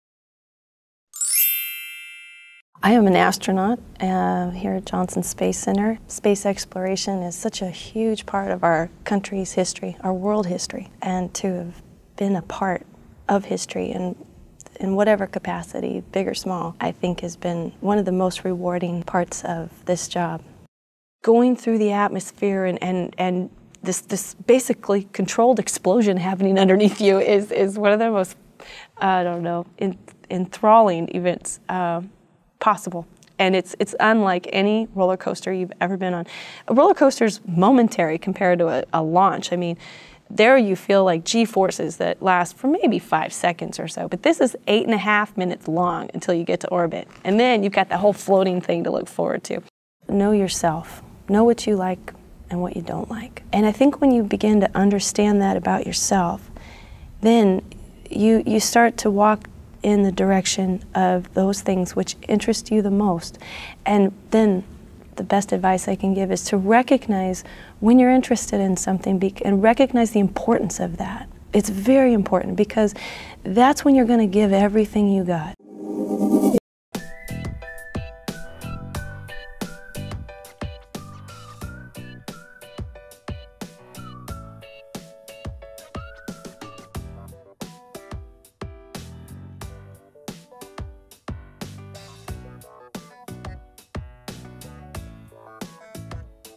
Astronaut | Tracy Caldwell Dyson | 60 Seconds
Watch Tracy Caldwell Dyson, Astronaut, describe “the list” and how she discovered that she wanted to be an astronaut. She explains how you can be both a scholar and an athlete during her undergraduate studies at Cal State Fullerton as a chemistry major and track and field competitor. She compares the excitement of "launch" versus "landing" and describes the thrill of being your own “satellite” during her three space walks. Be sure to click on her special messages in American Sign and Russian.